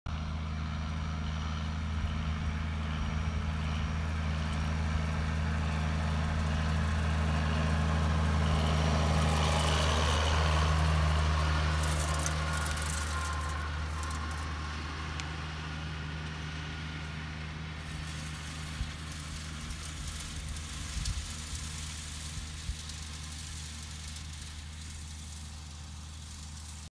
John Deere 6930 spreading with an Amazone sower
John Deere spreading fertiliser with gps guided sower.